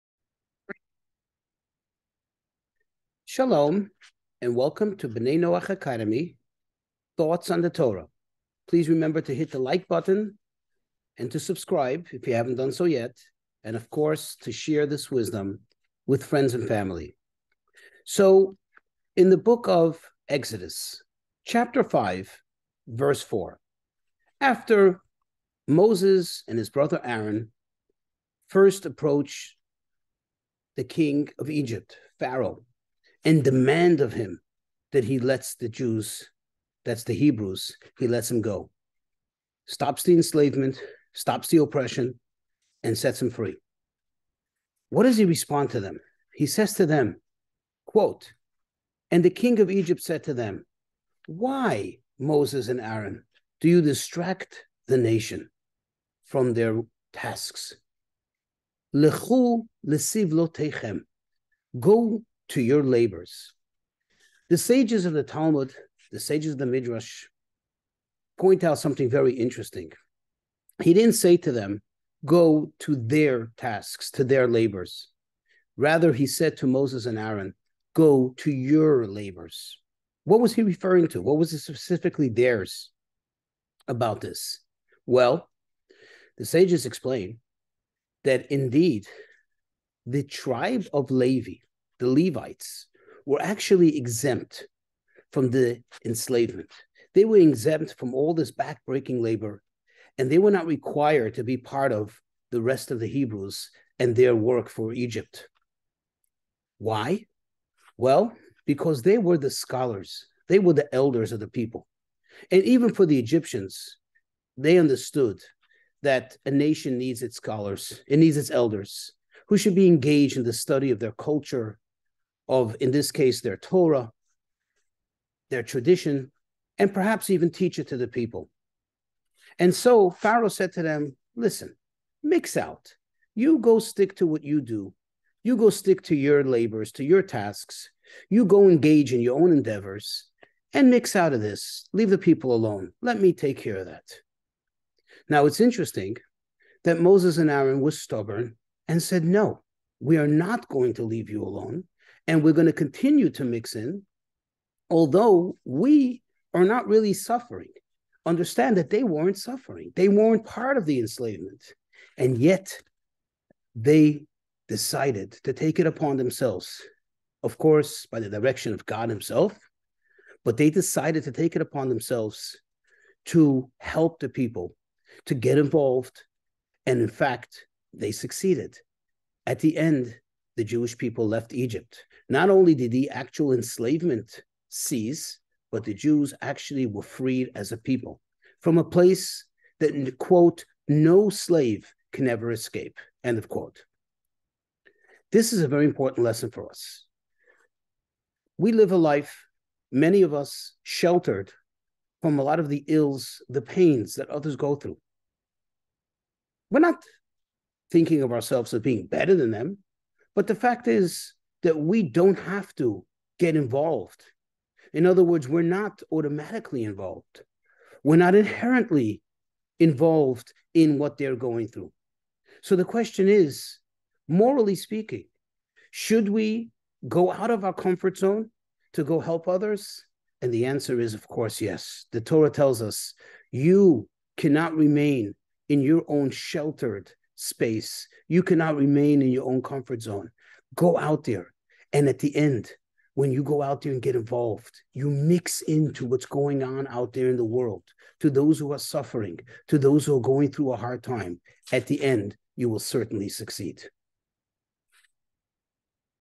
Is It My Problem? Do I Really Need To Get Involved? By: Rabbi Avrohom Brashevitzky
Many of us live a peaceful tranquil life, isolated from many of the ills of society. Question: should we leave our comfort zones to get involved? Is it really our problem, our calling?